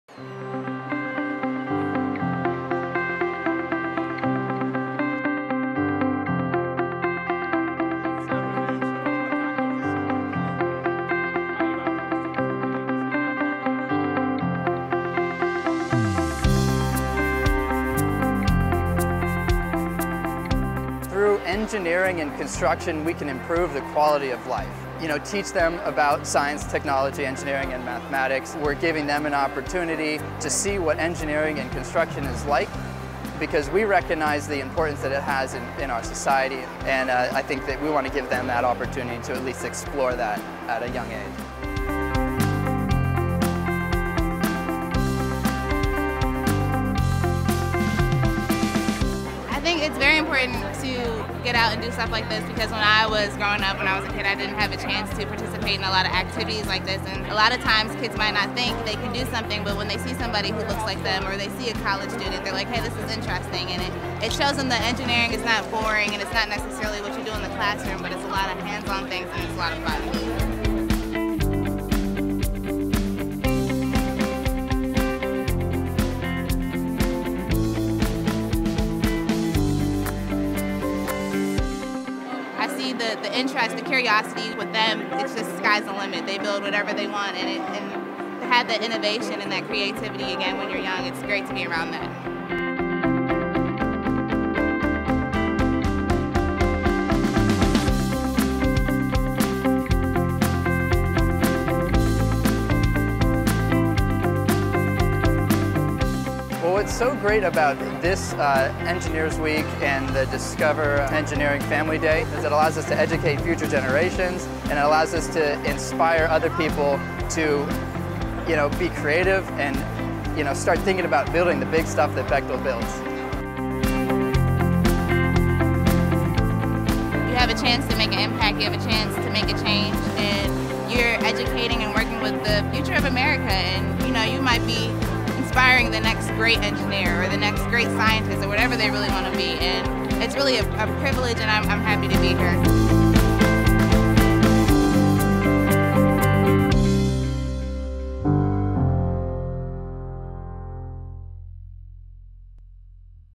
Discover Engineering Family Day
Looking to get kids interested in engineering? This informational video shows how exciting and effective hosting a Discover Engineering Family Day can be. This video is part of a larger resources volunteers can use to learn how to host an event like this. for more information.